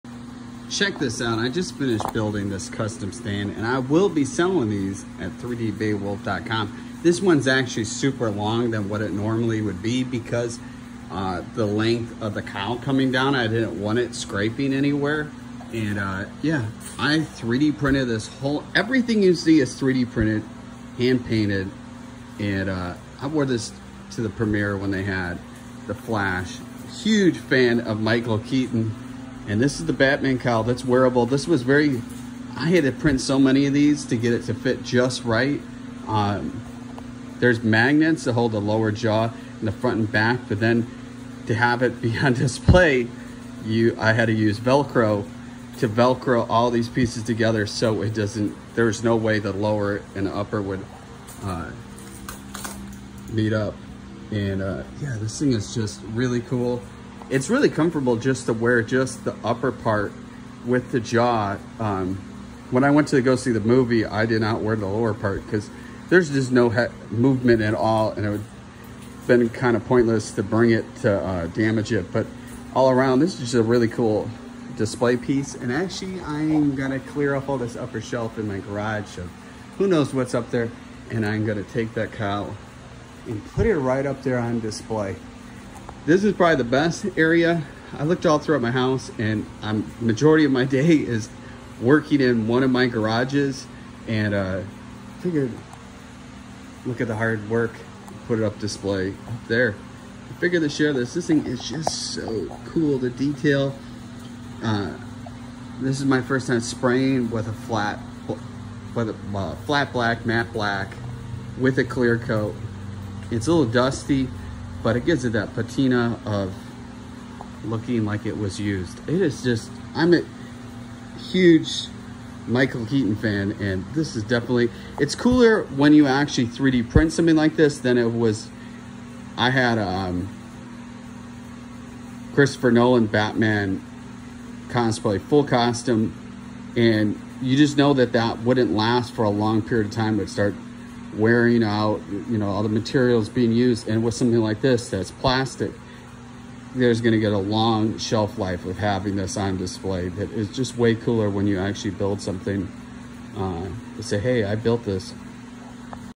DO3D 3d printed Batman Cowl
I 3d printed flash movie Batman cowl. Files are from DO3D. Was a lot of work but worth it and turned in to cool display.